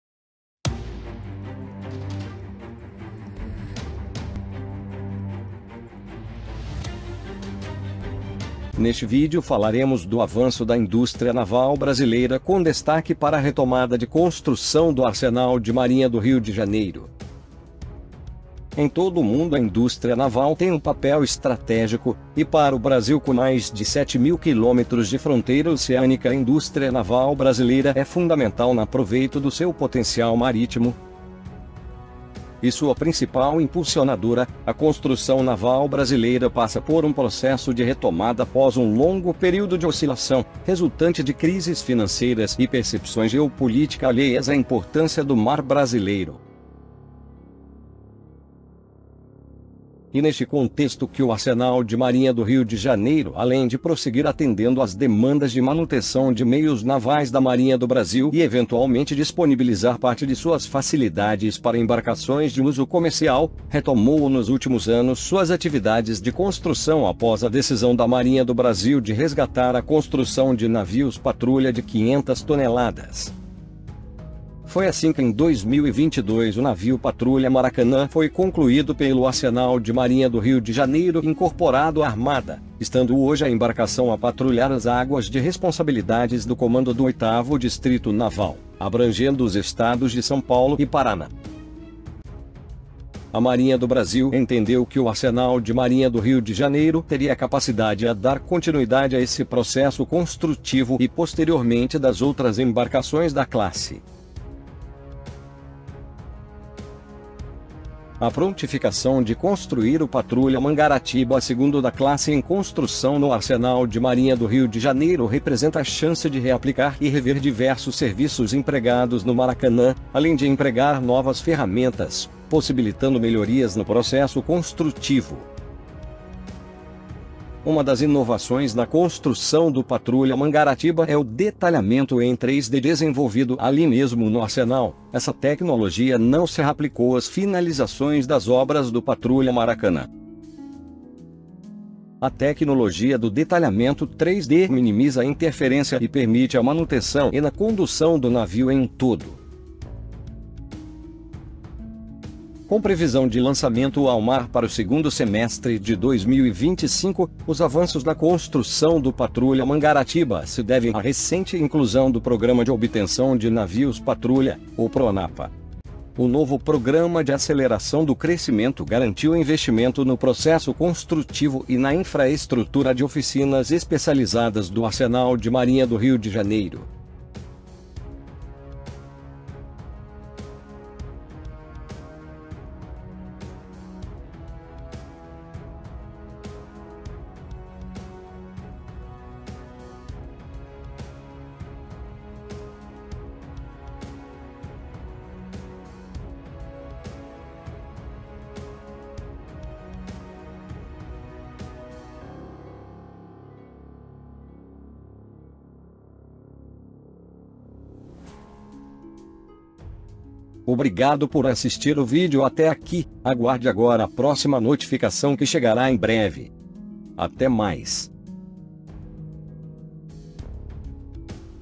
BRASIL avança em tecnologia para CONSTRUIR NAVIOS DE GUERRA.
No vídeo de hoje falaremos da nova tecnologia nacional que o brasil implantou para construir futuras embarcações de combate.